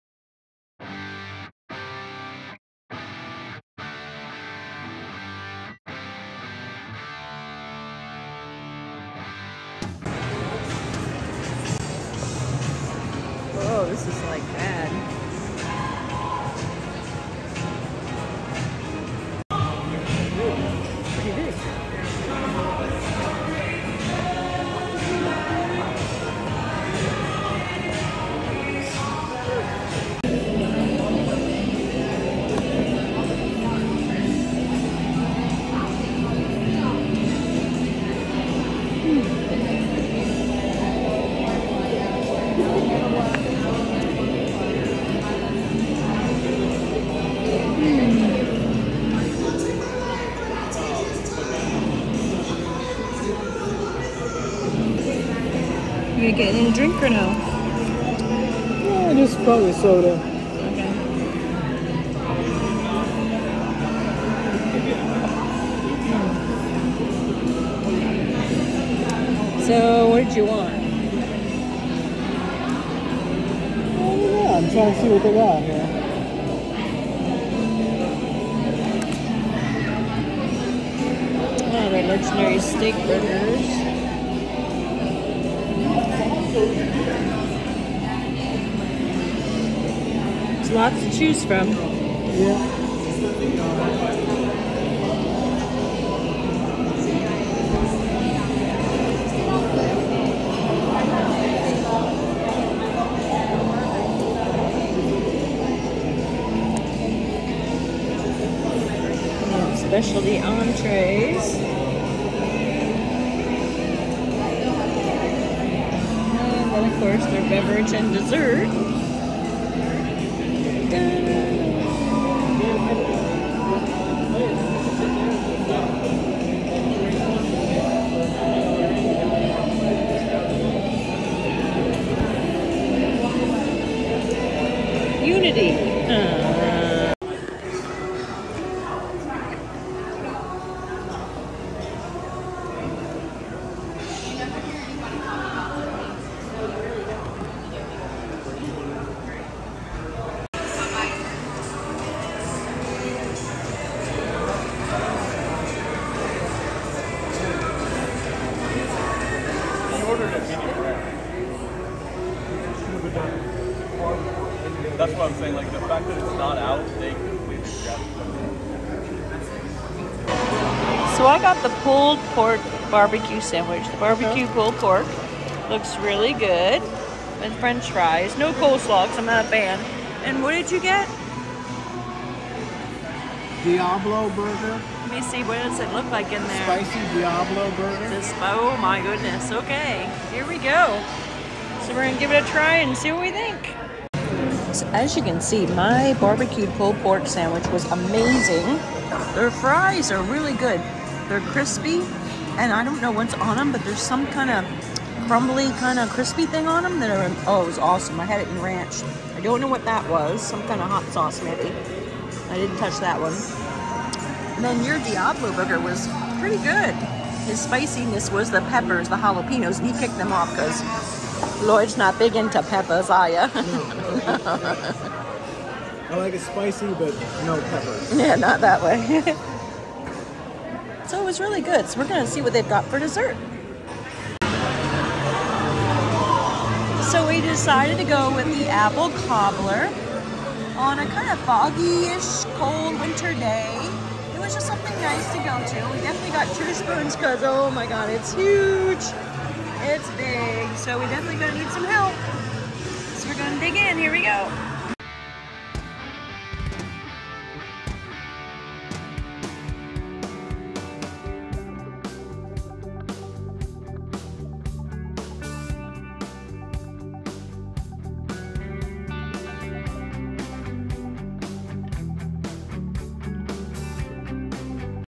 Dining at the Hard Rock Cafe in Foxwoods Casino
We got lucky and found a Hard Rock Cafe in the Foxwoods Casino in Connecticut.
This is a quick review on what we ordered and just a glance through all the memorbilia on the walls.

0:00 Intro
0:33 Menu
2:50 Food Arrived
3:56 Dessert